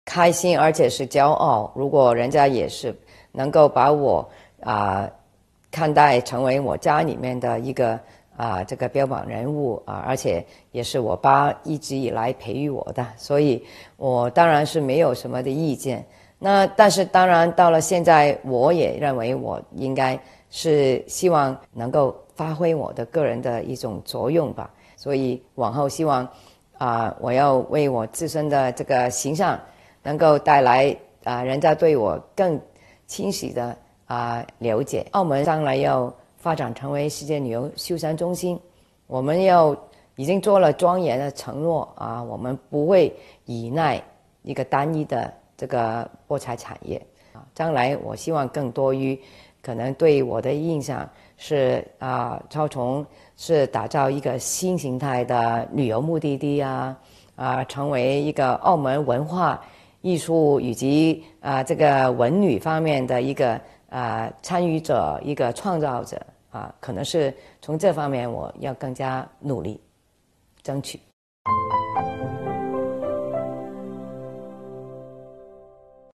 【两会声音】何超琼：将澳门打造成新形态的旅游目的地
港区全国政协委员何超琼在近日接受中新社专访时表示，父亲何鸿燊一直以来培育自己，如果外界将她看成家里的一个标榜人物，自己当然是非常开心且骄傲的。何超琼也表示，澳门将来要发展成为世界旅游休闲中心，不会依赖单一的博彩业。她希望将来外界对她更多的印象是，超琼成为澳门文化艺术以及文旅方面的参与者和创造者，将澳门打造成为一个新形态的旅游目的地。